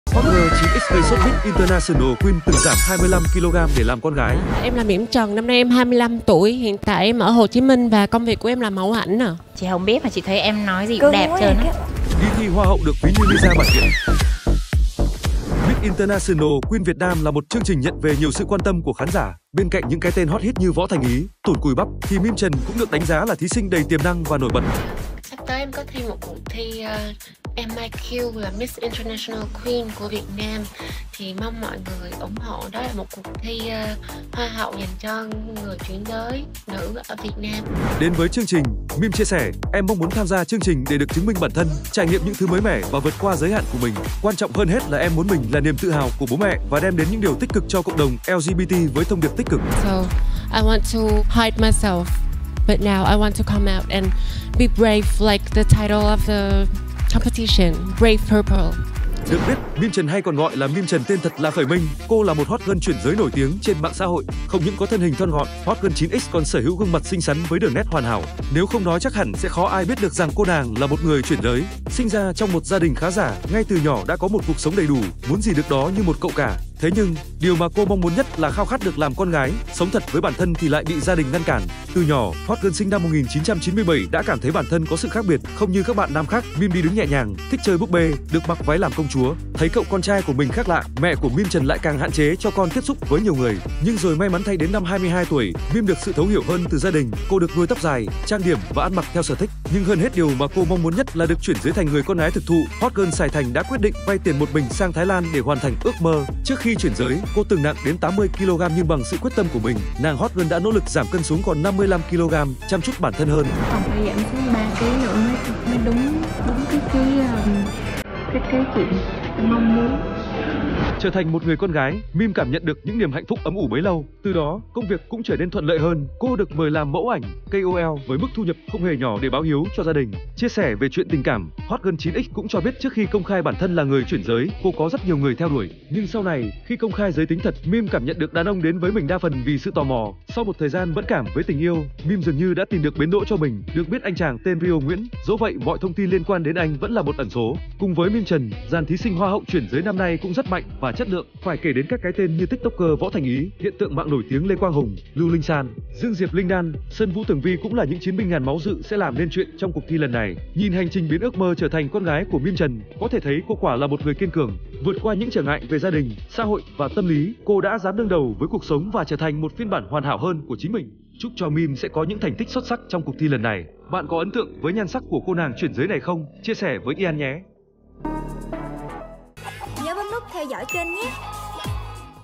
Hot girl chuyển giới gây sốt CĐM: Mỹm Trần nổi bật tại Miss International Queen Vietnam 2023
Hãy theo dõi Yan trên:
✔️Youtube Yan News :